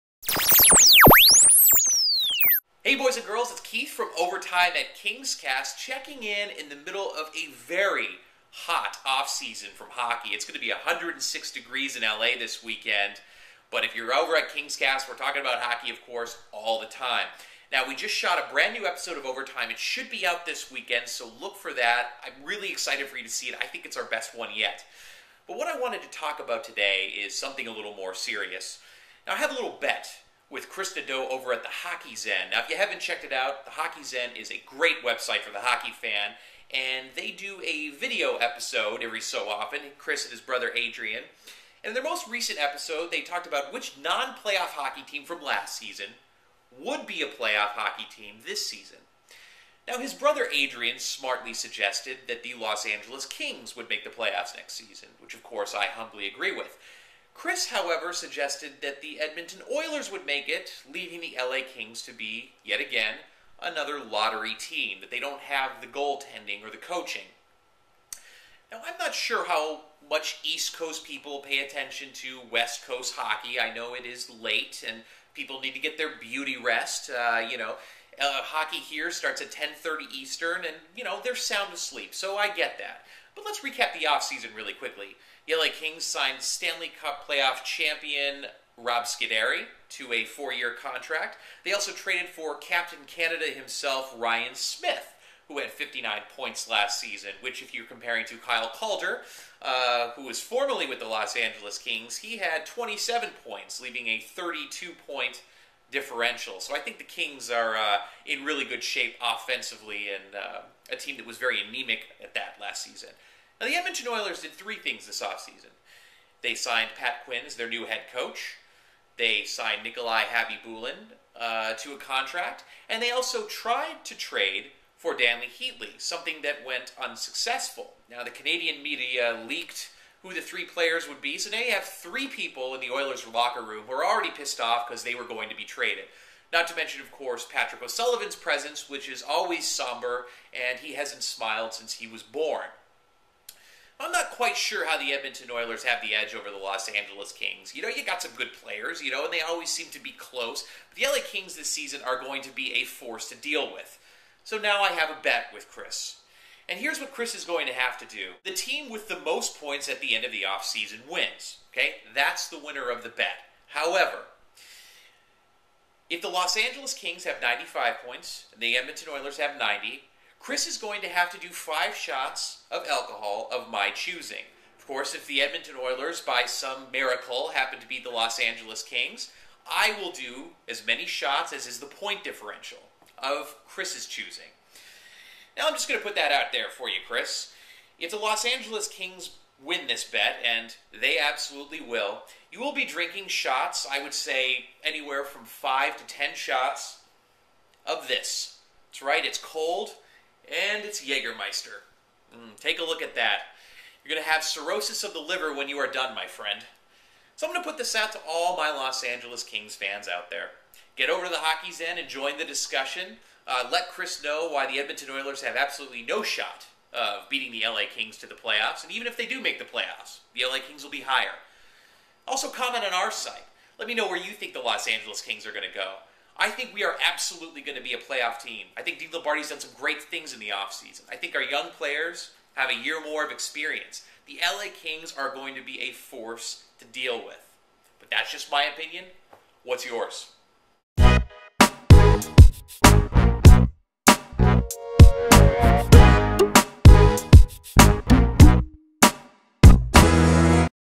Episode 7 - The Bet
Will the Edmonton Oilers end with more points than the LA Kings next season?  Keith doesn't think so.